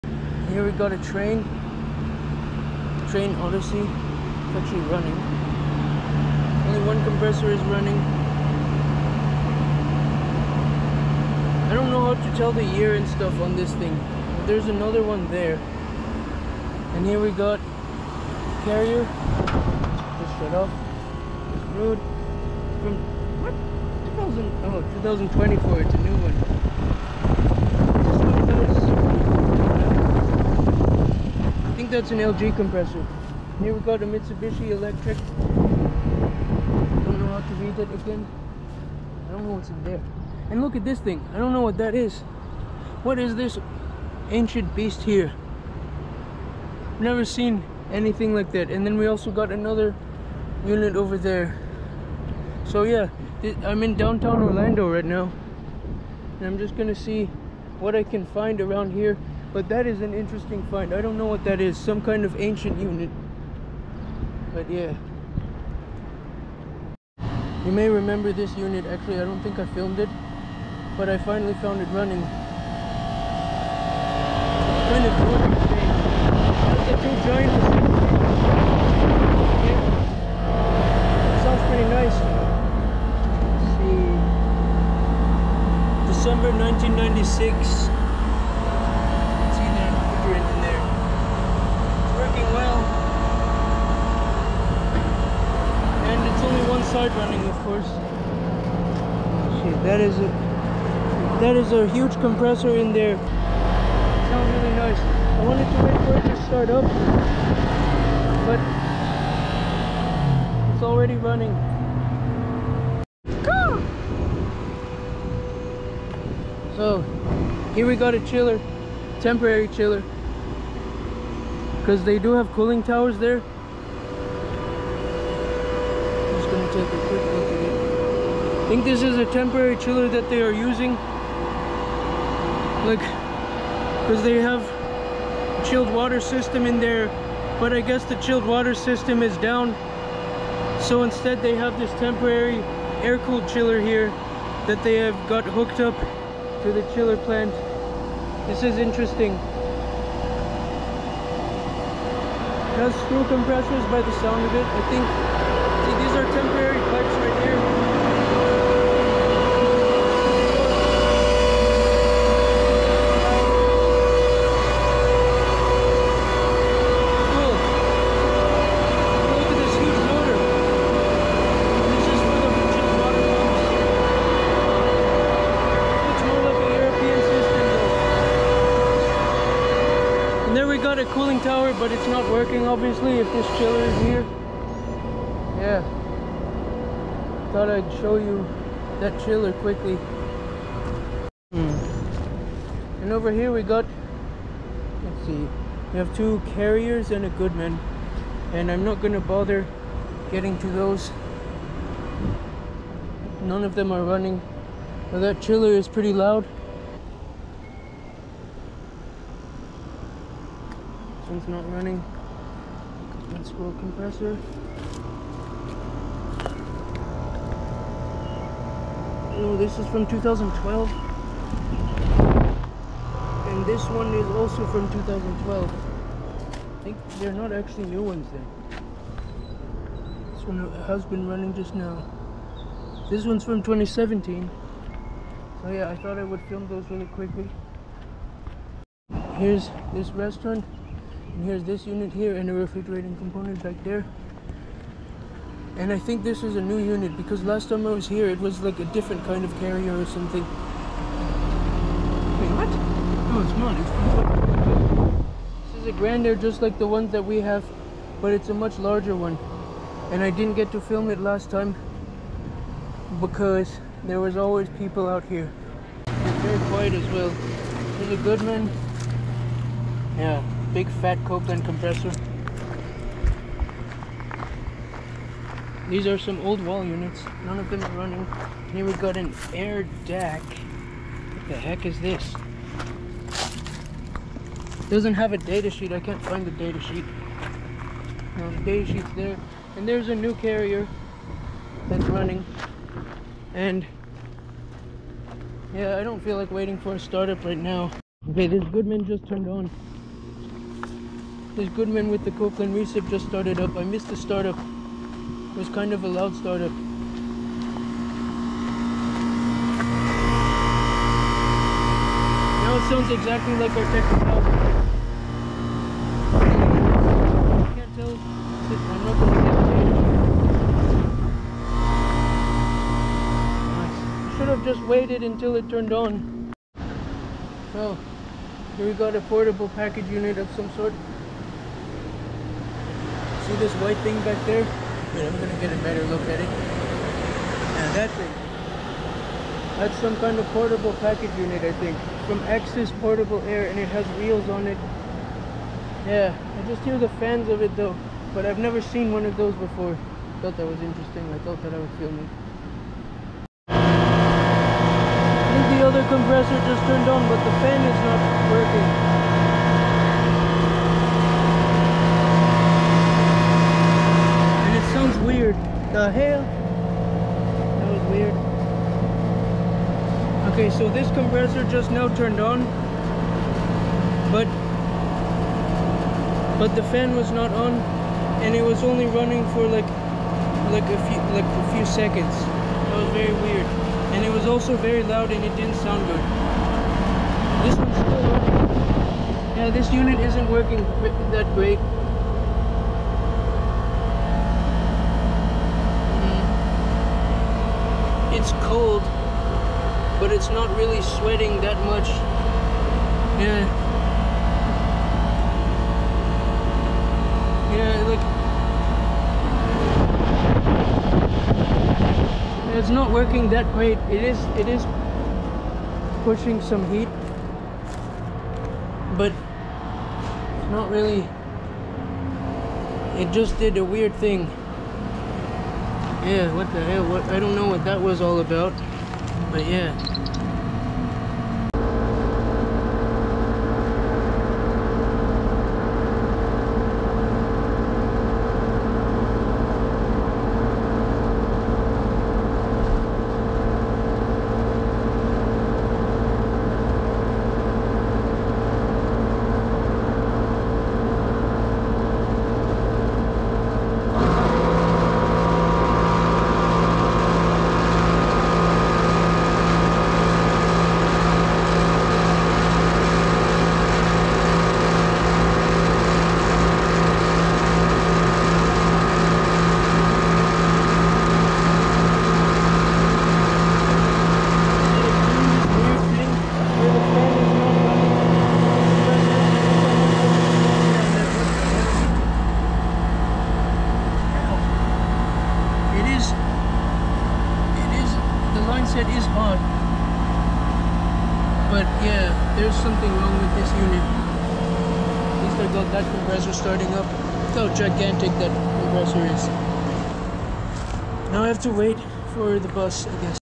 Compilation: HVAC around downtown Orlando!
I took a walk downtown on a hot day. Some of these have been filmed already by my brother @carlosilva1158 .

Starting and ending with the old Trane commercial system, which was broken. One side seemed to be running constantly but not removing much heat for its size. The other side kept turning on every 10-15 minutes, but when it did, the fan was not running and the compressor was only on for a few seconds. It also sounded weird. I wonder if that thing is at the end of its service life.

Key moments:

0:00- newer Trane commercial systems
0:18- Carrier Comfort, 2024 Ruud, and Mitsubishi mini split
0:40- unknown old packaged unit
1:08- 1996 Trane with giant Climatuff running
1:53- temporary portable air-cooled chiller (and other units)
3:27- 3 good men
4:02- large Grandaire
4:26- Rheem Classic, Goodman, old wall units, and more
5:28- weird portable packaged unit
5:53- the old Trane being weird
7:12- the old Trane being weird again, but with startup!